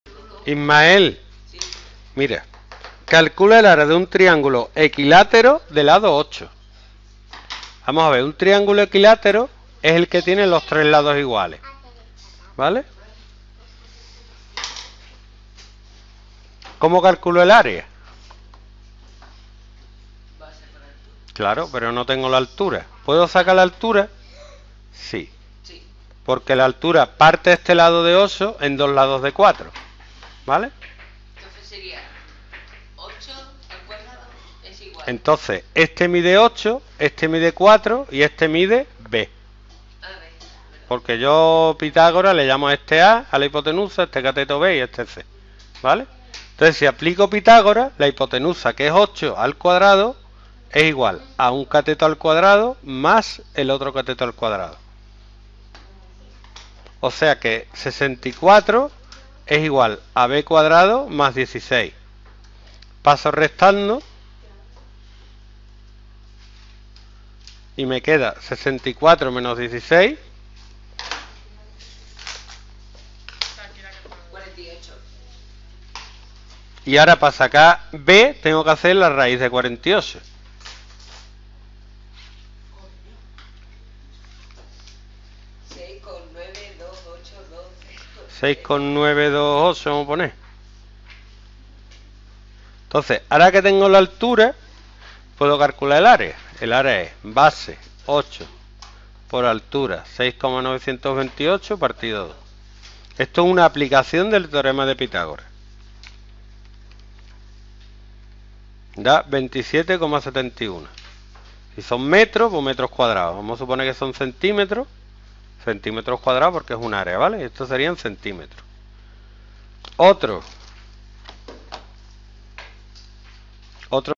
Calcular área de triángulo equilátero Matemáticas 2º ESO Academia Usero Estepona
En este ejercicio calculamos el área de un triángulo equilátero sabiendo la longitud de sus lados:
1) Dividimos el triángulo equilátero en dos partes por su altura.
2) Tomamos uno de los dos triángulos rectángulos que resultan al dibujar la altura.
3) De ese triángulo conocemos la hipotenusa, que es el lado del triángulo, y el cateto de la base, que es la mitad del lado.
4) Aplicamos el teorema de Pitágoras y calculamos así, el otro cateto, que es la altura.
5) Ahora que ya sabemos base y altura, podemos calcular el área del triángulo.
Teorema de Pitágoras y semejanza UD 08